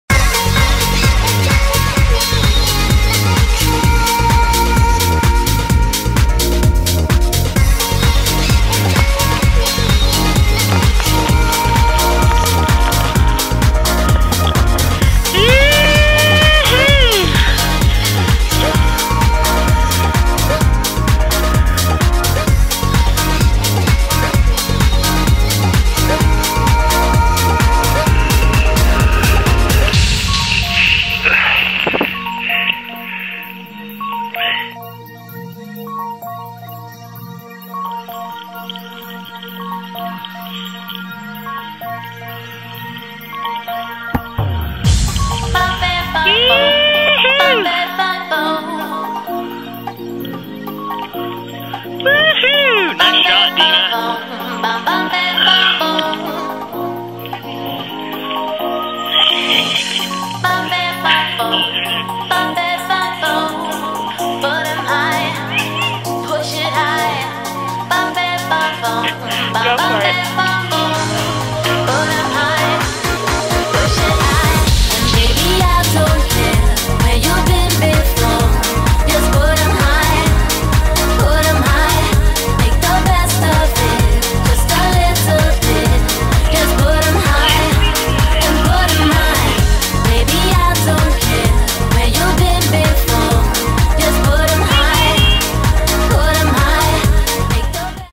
Aspen Mountain Powder Day Skiing Bomb!
Powder day on Aspen Mountain in Colorado...host your next meeting with us